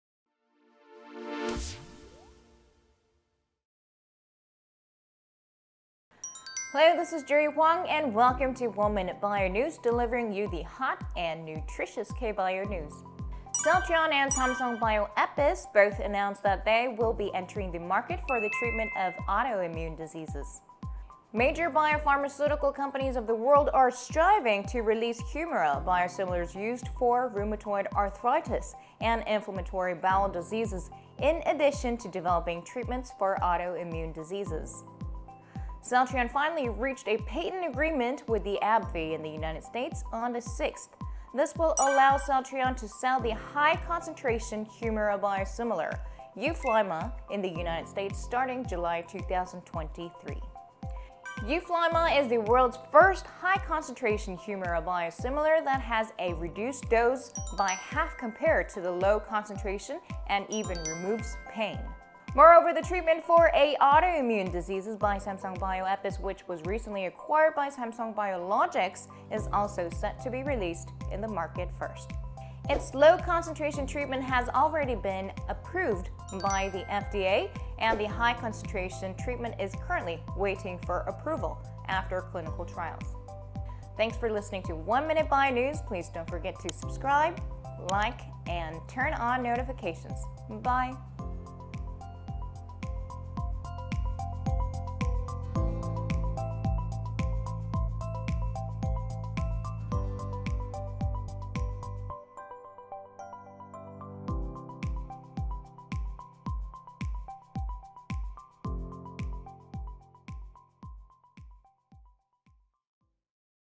[Bio NEWS] Celltrion and Samsung Bioepis to enter global autoimmune market
1Minute BIO NEWS #19

Celltrion and Samsung Bioepis to enter global autoimmune market